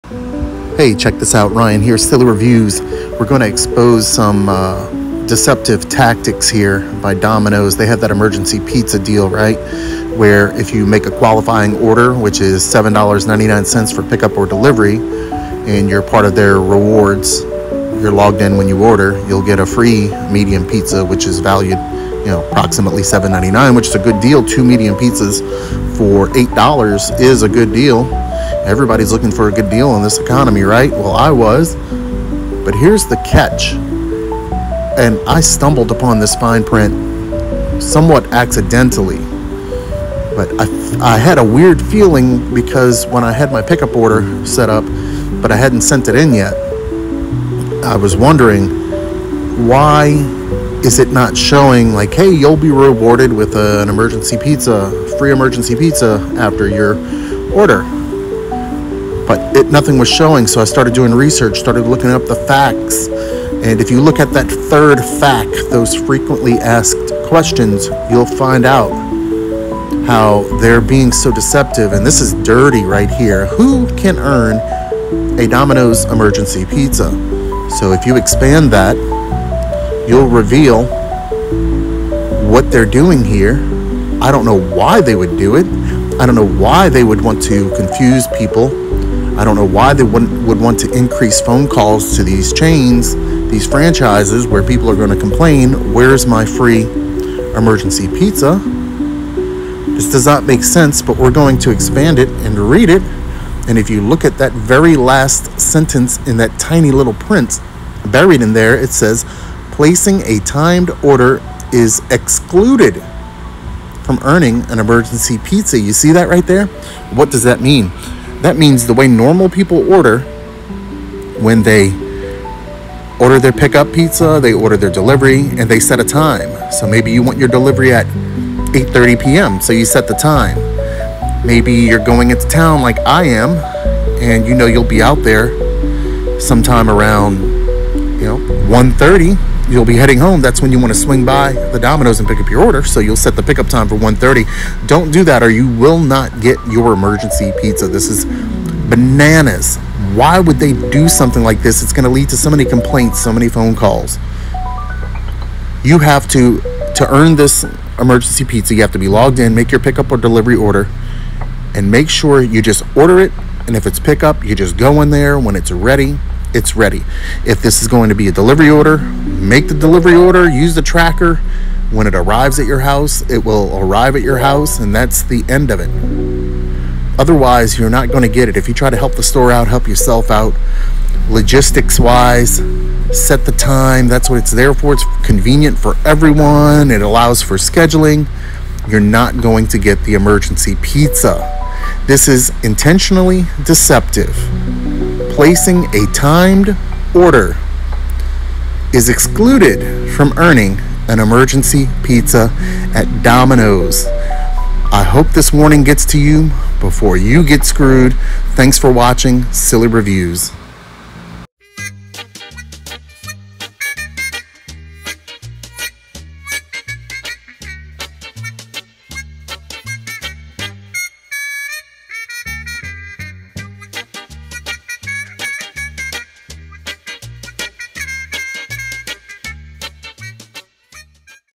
Dominos Emergency Pizza - Timed Orders DO NOT QUALIFY!
Please watch this video so you can avoid being deceived and tricked by Dominos and their fine print which will disqualify you from receiving the free “Emergency Pizza” they promise in their latest promotion.
Bitcoin: 3MoanBGNDLQ3mdjH1e4u2Di7Tuzx1vPydH
Litecoin: MFFceWzvqx5KFNjs7Js85cs6naN5RD4ZYp
Ethereum: 0x1f42C49Fc6C18a0BD7e1007e078a46F77283f65e